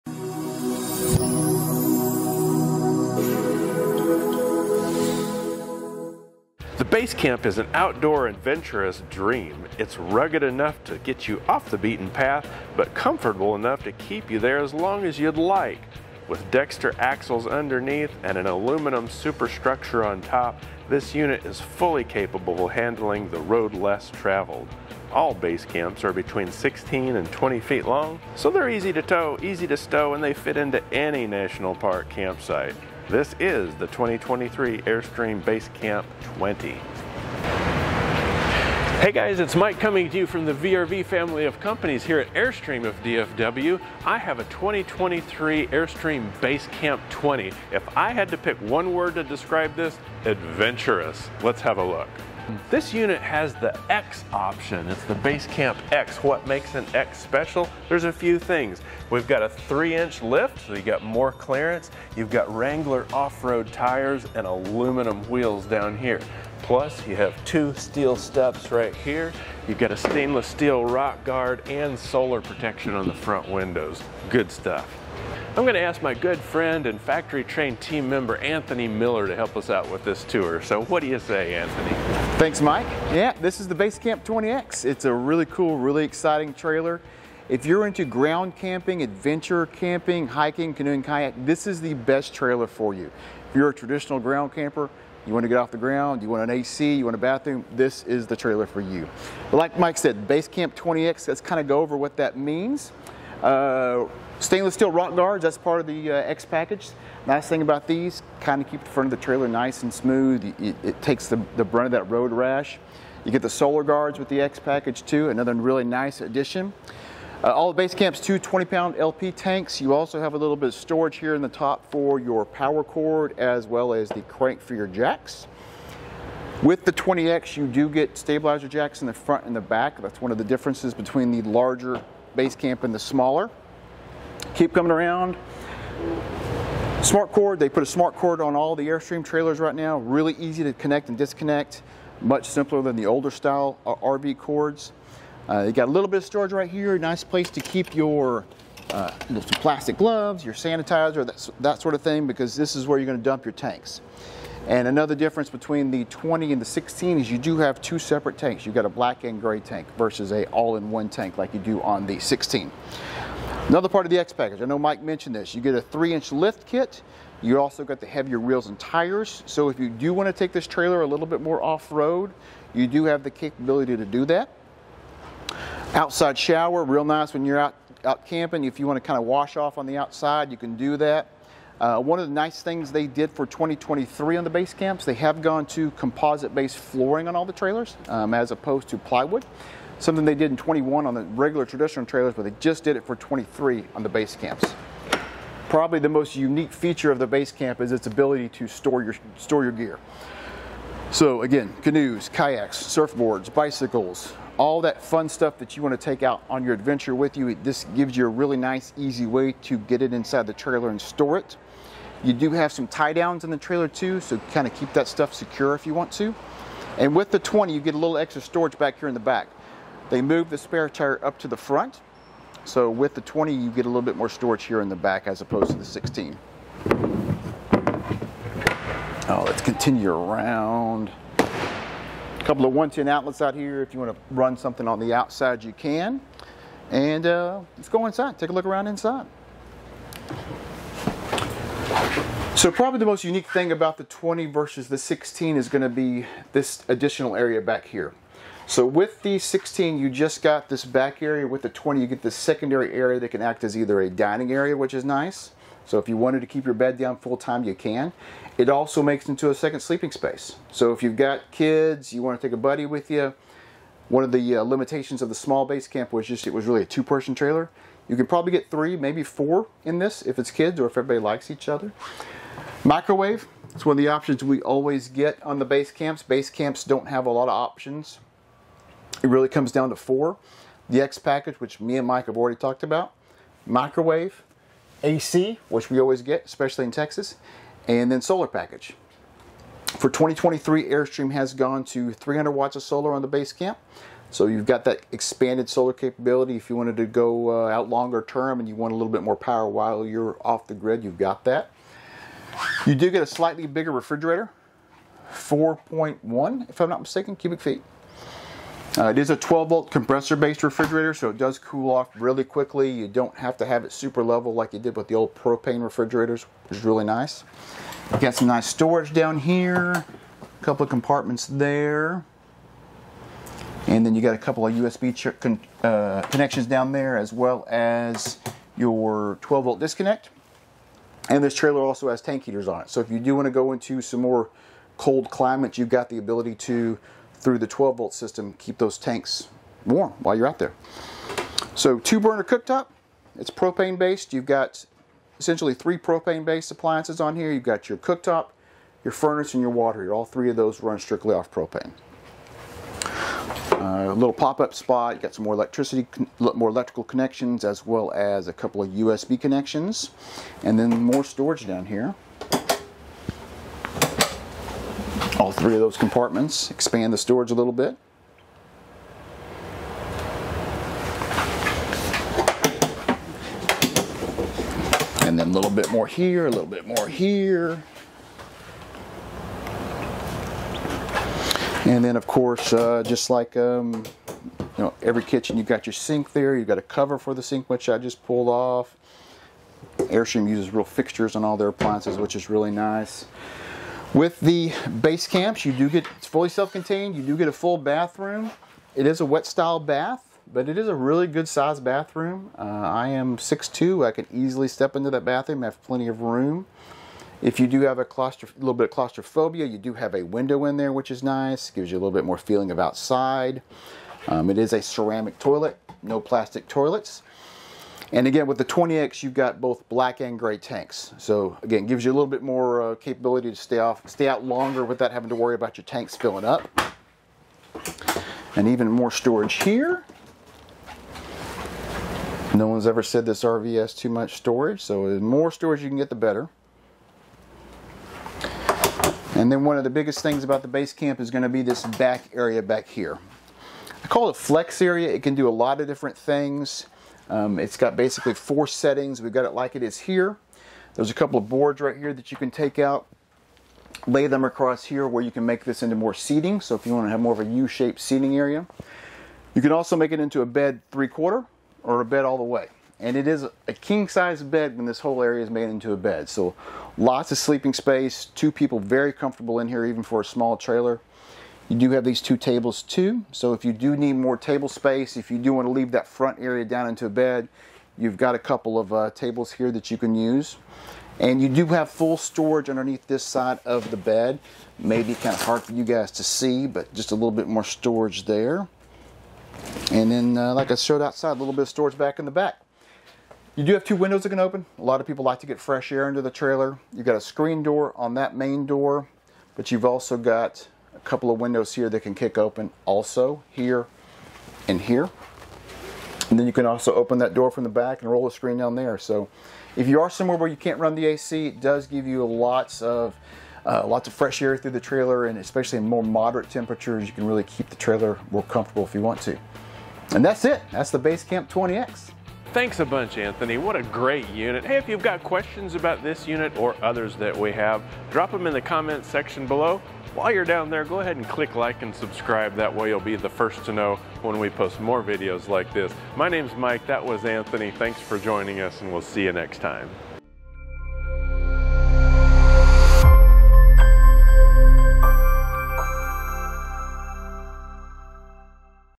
RUGGED and Ready for Every Adventure! | 2023 Airstream Basecamp 20X
CHECK OUT:
2023 Airstream Basecamp 20X

SPECS:
Length: 20' 2"
Width: 7' 9.5"
Dry Weight: 4,300 lbs
Gross Weight: 5,200 lbs
Fresh Water Capacity: 23 gals
Gray Tank Capacity: 28 gals
Black Tank Capacity: 21 gals
Sleeping Capacity: Up to 4

TIMESTAMPS:
00:00 - Intro
00:07 - Specs
00:52 - X Package
01:22 - Exterior
03:57 - Interior
04:07 - Dinette
05:02 - Galley
06:34 - Bathroom
07:20 - Bed/Flex Area
08:42 - Outro